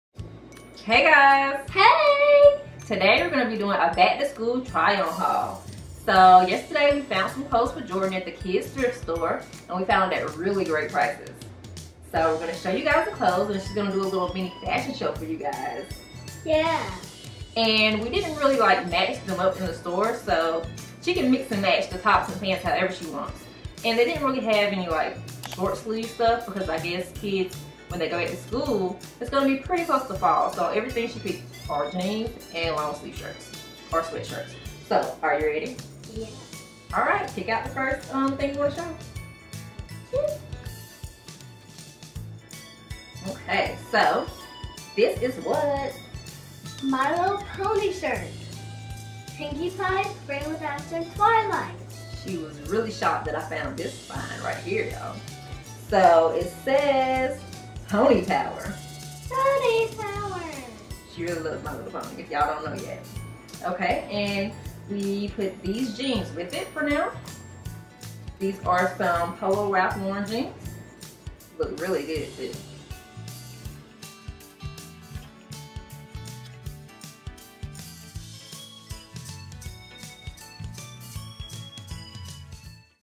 Back to School Shopping Fashion Show | Shopping for Kids
We found some awesome kids clothes for Jordyn for a great price at a kids thrift store.

KiaFocused
Only1Jordy